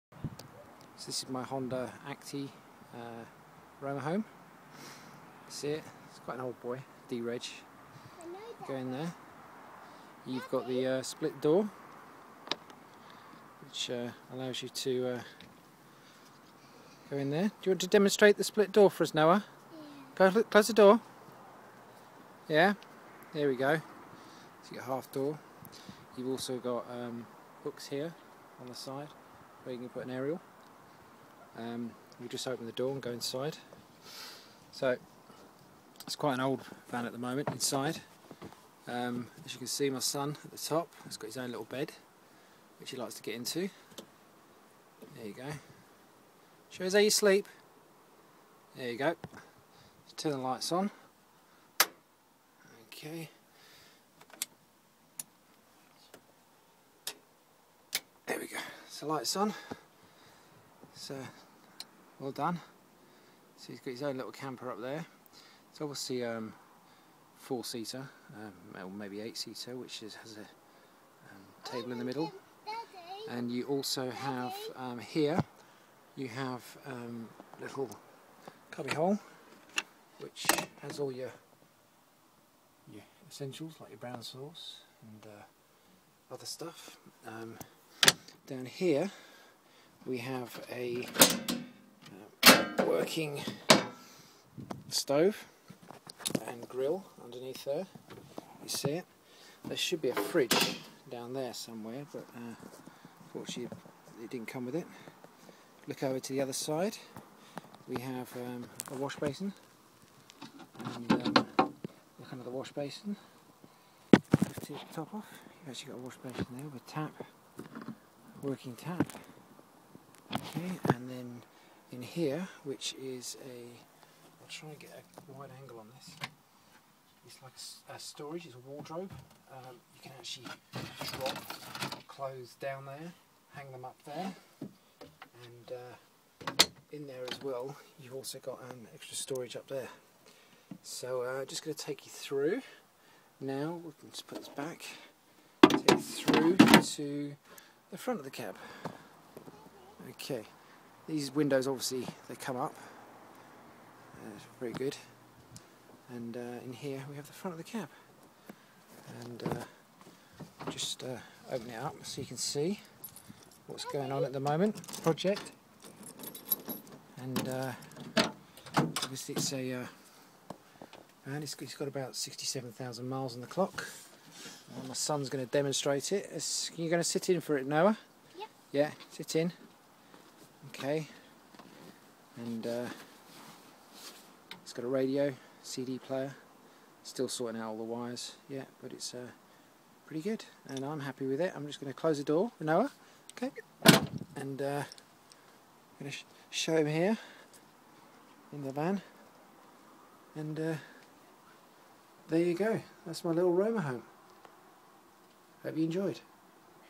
Honda Acty Romahome
This is my 1986 D reg Romahome I just bought. I have spent one solid week cleaning it replacing the carpets getting the electrics working etc. It's my first motor home and still needs a lot of work but it will be an on going project.